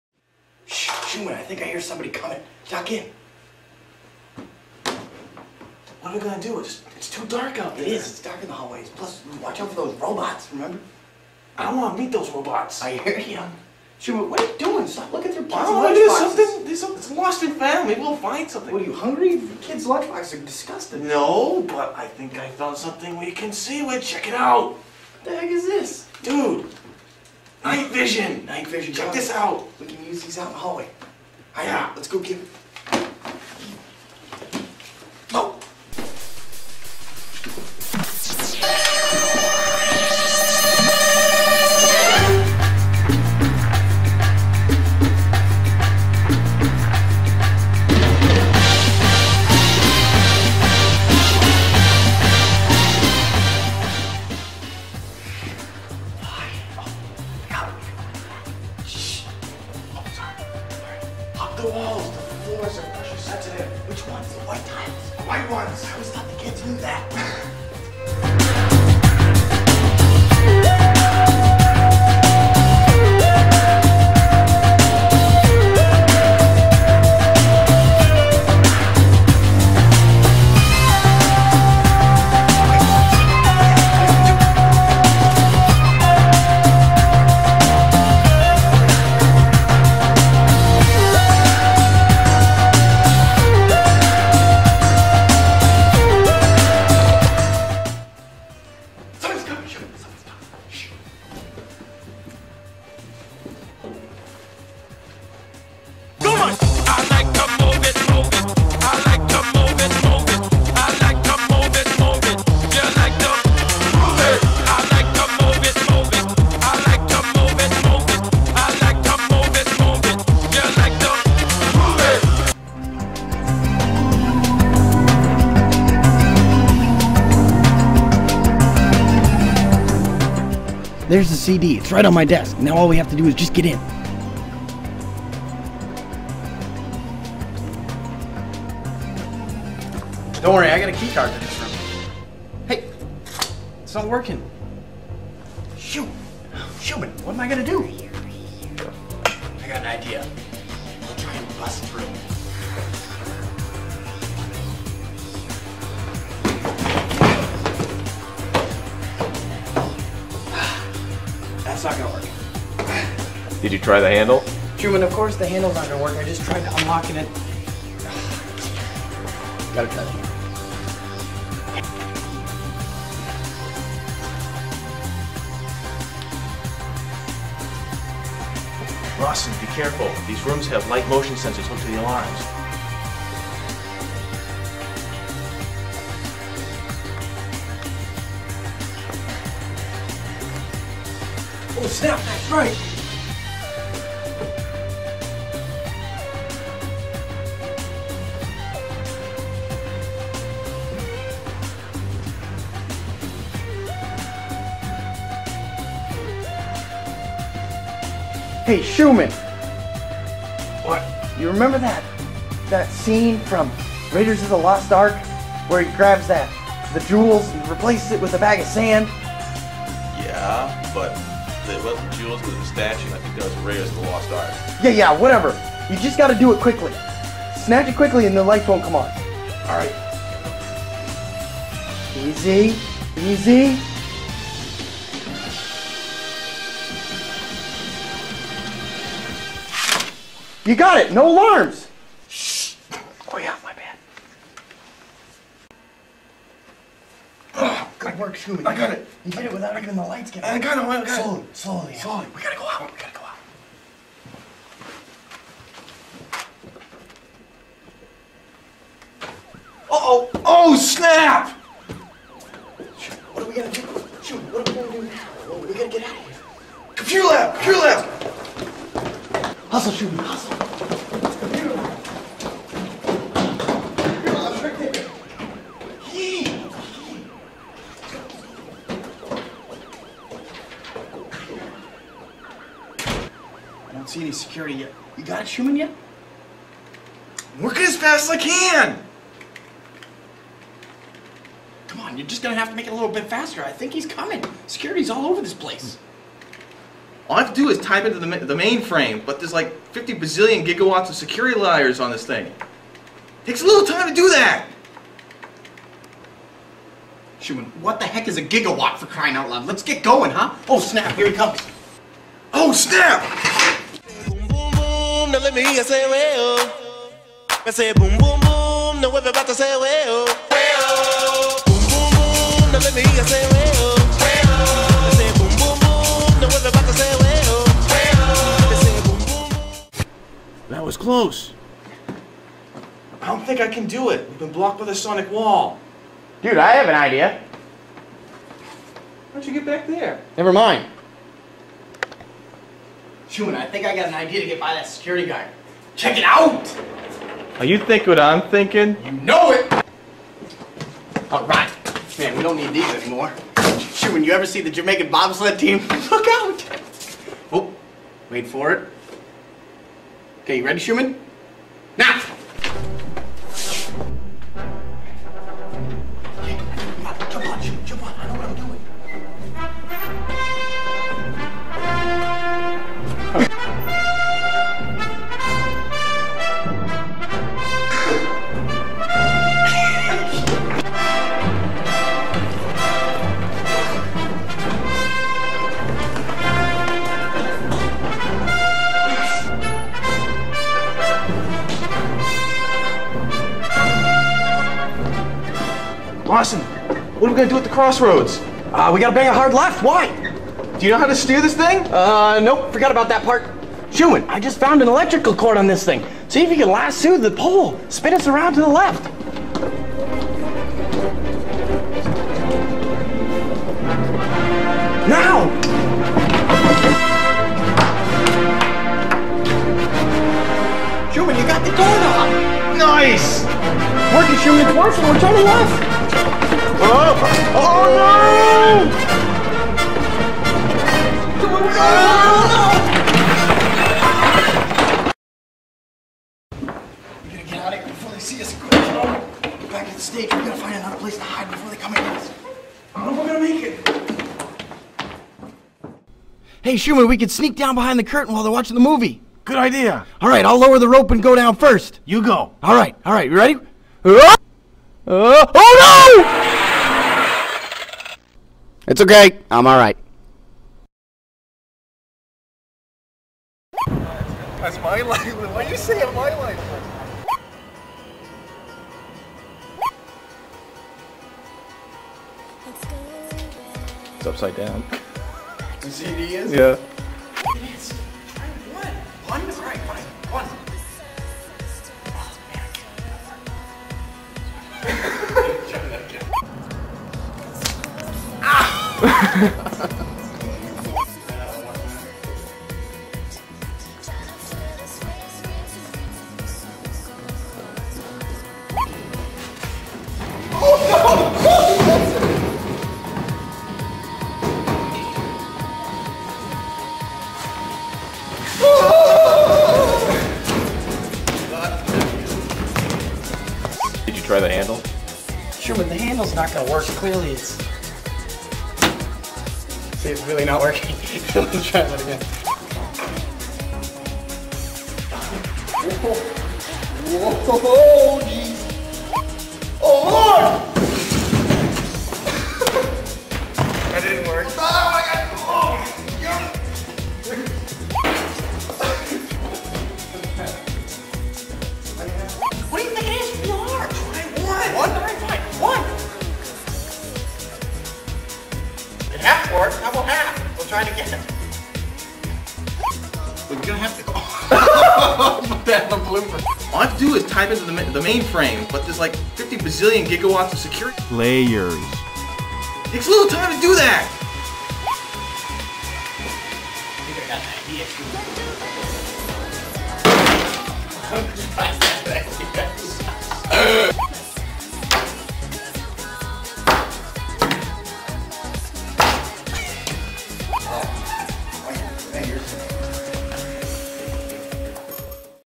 Oh Snap! - (Part 2 of 2)
A short skit designed as an introduction to an end-of-year slide show for elementary level students.  In this movie, two teachers need to find a way into their school after hours in order to obtain the end-of-year movie.